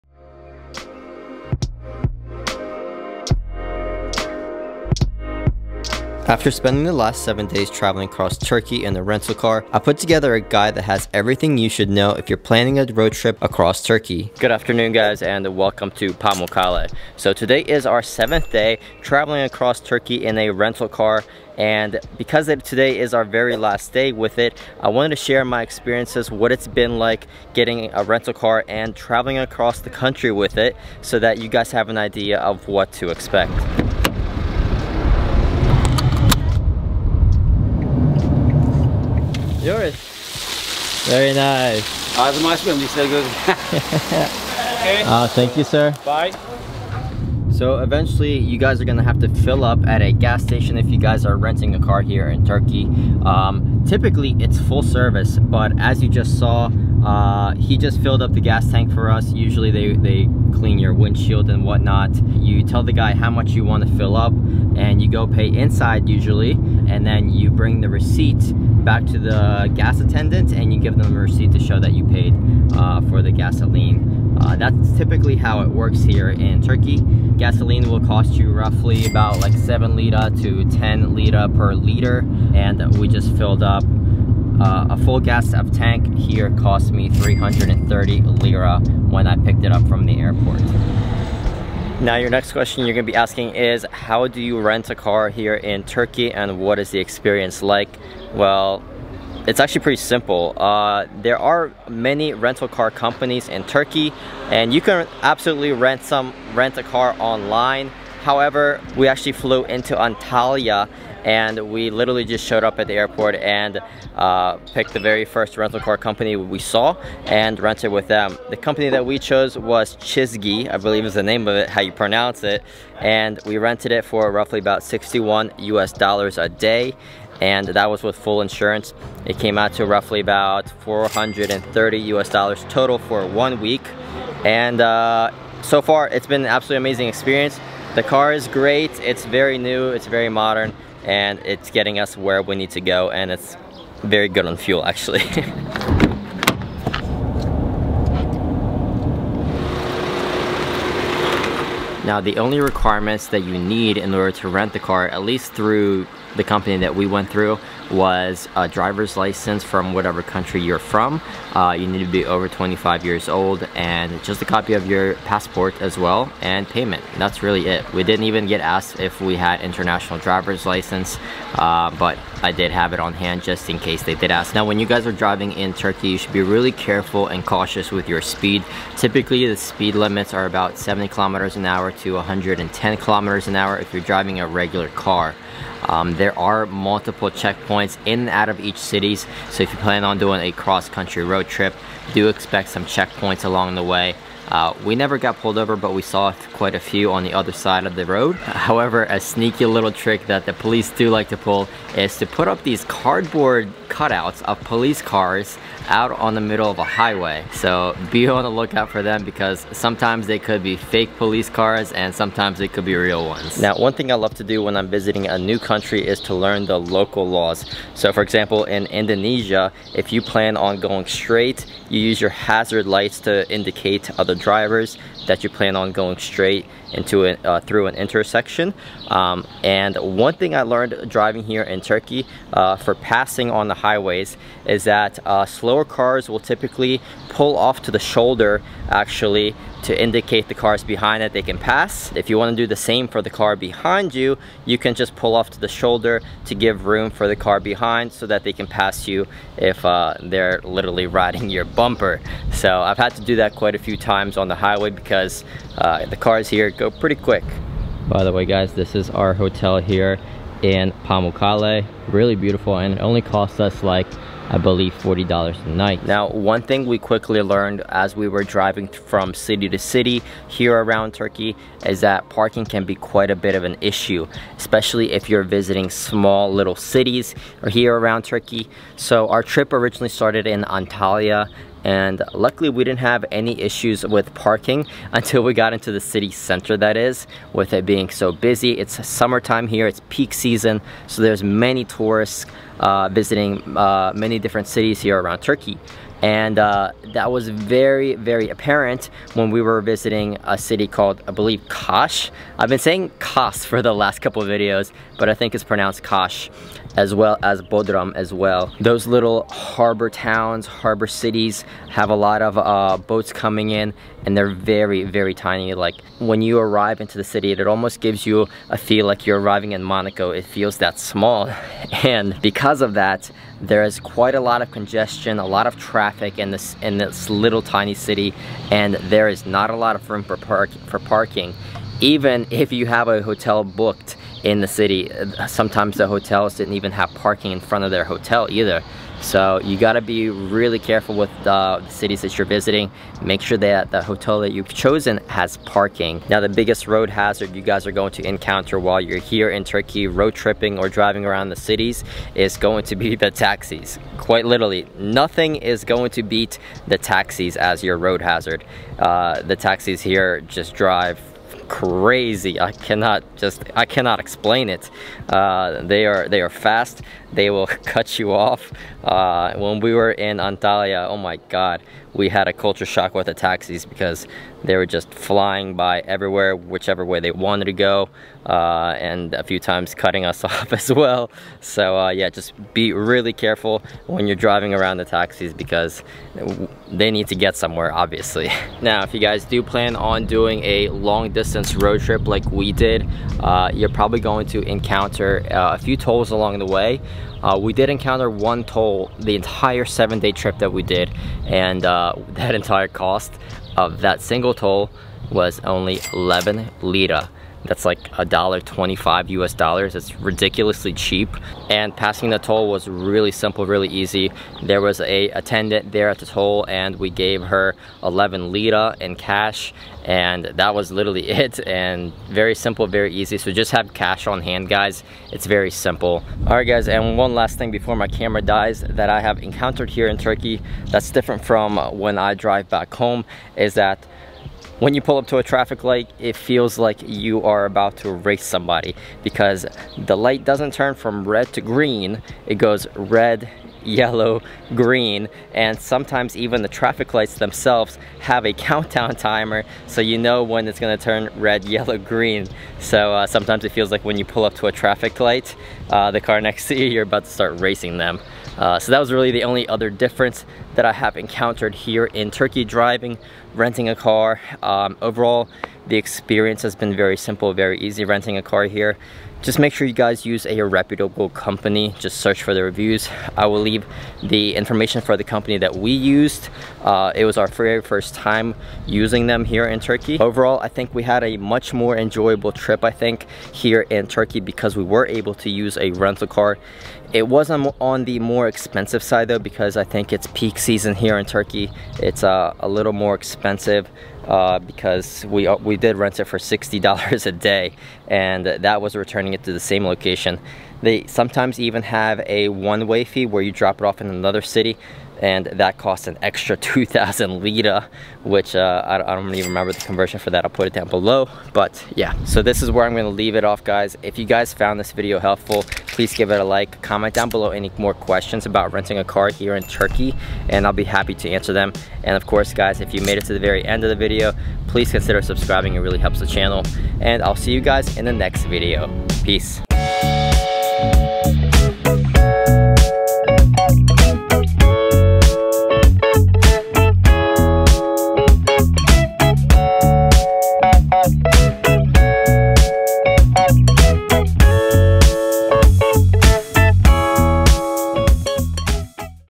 Renting a Car in Turkey as a Foreigner (what you must know)
After spending 7 days driving across Turkey I wanted to share a full guide on what its like renting a car in Turkey as a Foreigner. These are the top things you must know if you plan to rent a car in turkey, as a few things are different for foreigners. Driving on the highways of turkey I learned a few important things regarding; tolls, police, parking, rental process, who to rent from, and how much will it cost you. If you have any more questions regarding the rental process drop your questions below. If you want to know the company we rented with it was Cizgi